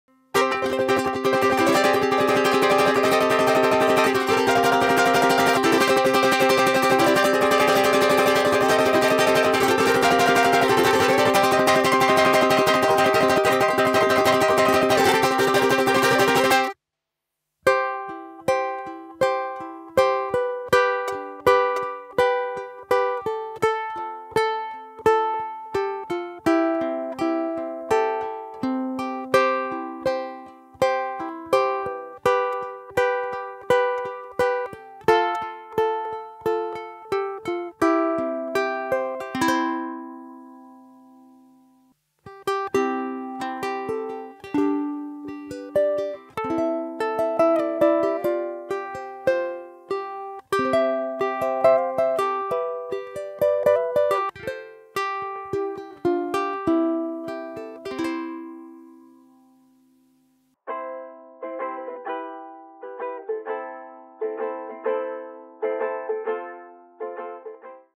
Flight Victoria Tenor Ukulele Sound Demonstration
In this video super talented Spanish ukulele player @DaniUsero demonstrates the sound of Victoria Tenor ukulele from Flight Princess series.

The Victoria is worthy of its royal title.  The combination of the solid top spruce and resonant acacia back and sides gives this instrument a lively, rich tone that will truly have you feeling regal.  With an armrest and unique round cutaway for extended fretboard access, the advanced player will enjoy a comfortable and pleasurable playing experience. The ebony fretboard outfitted with Japanese fluorocarbon strings is crowned with a stunning slotted headstock.  With all of its high end attributes and striking features, Princess Victoria has everything an advanced player could ever desire.